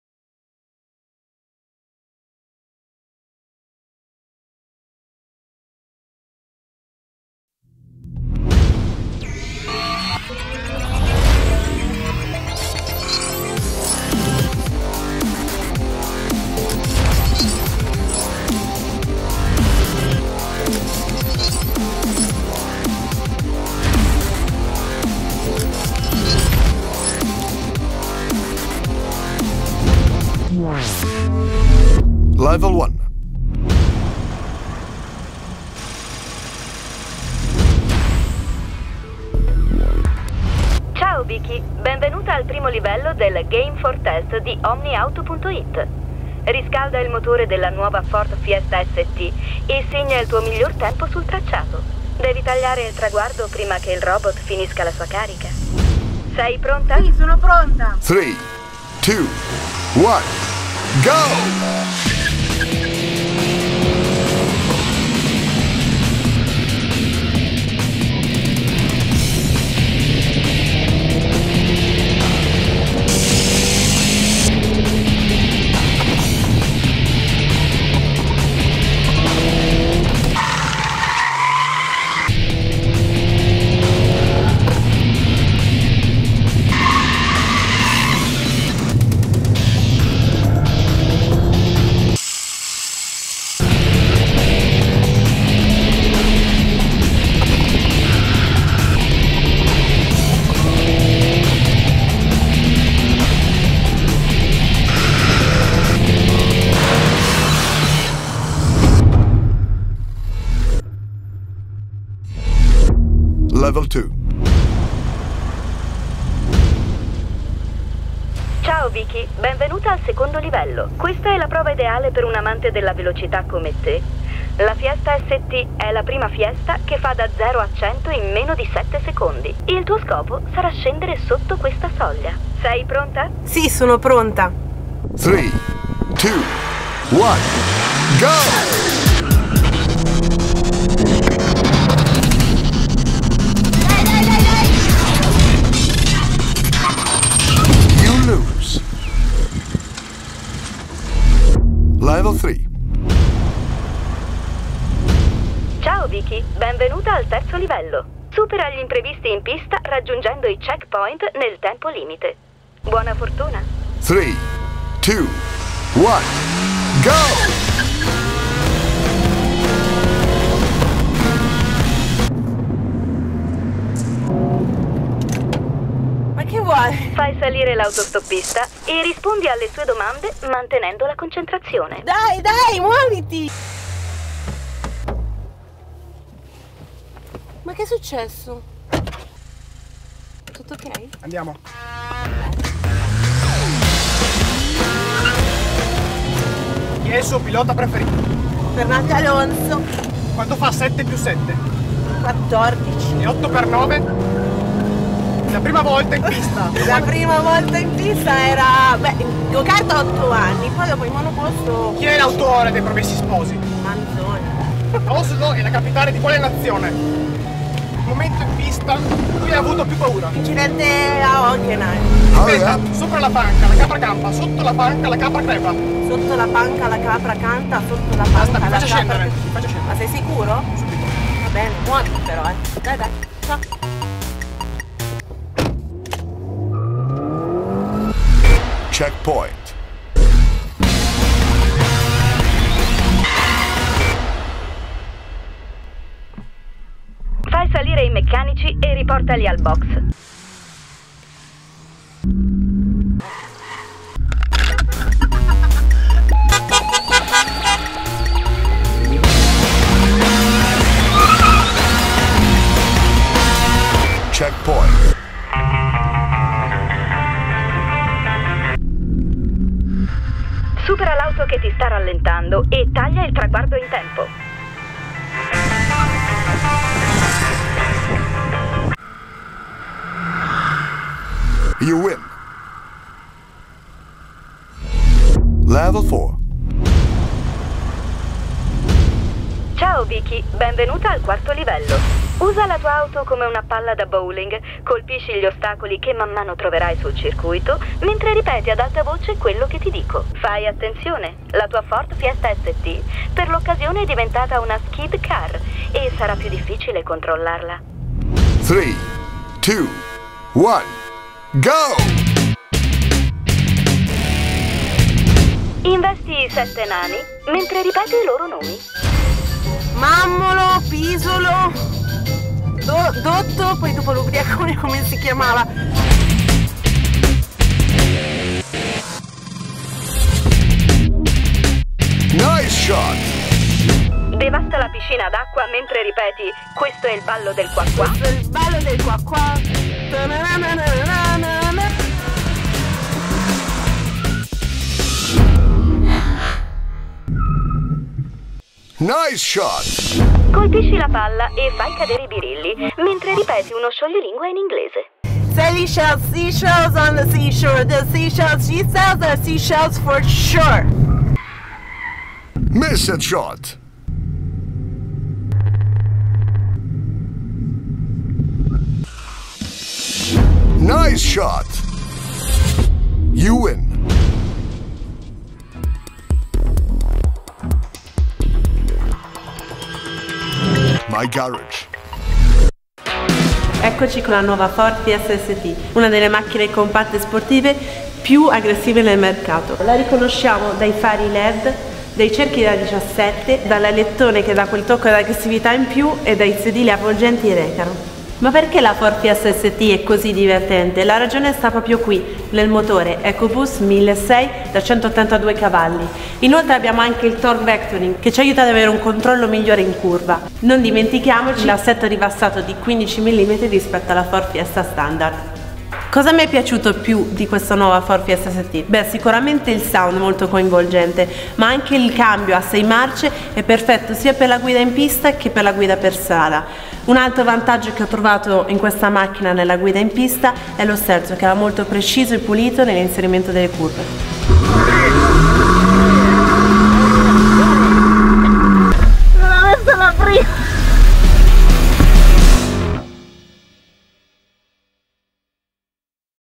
Ford Fiesta ST e Vicky Piria nel Game4Test
Prendete una Ford Fiesta ST, un pilota professionista di Formula 3 e una pista... Probabilmente vi state già pregustando le immagini di un hot lap o comunque di una prova di velocità, magari con delle rilevazioni strumentali particolarmente accurate... Ma siete fuori strada perchè in Redazione questo test lo abbiamo voluto diverso e per organizzarlo ci siamo ispirati al magico mondo dei videogames.

Ecco perchè si chiama Game4Test. Di cosa si tratta? Volendo essere sintetici possiamo definirlo come una prova su pista strutturata a livelli, come in un vero e proprio videogioco, in cui il pilota mette alla prova sia la propria abilità che le caratteristiche dell'auto "lottando" contro limiti di tempo, trabocchetti e cimentandosi in evoluzioni al volante ai limiti dell'assurdo.

Protagonista al femminile di questo Game4Test è Vicky Piria: giovane, brava e sexy, la pilota che sogna la Formula 1 ha spinto al massimo la nuova Ford Fiesta ST sul circuito di Modena, per superare i quattro livelli che abbiamo preparato per lei... Non vi vogliamo anticipare nulla di più e allora vi lasciamo al video.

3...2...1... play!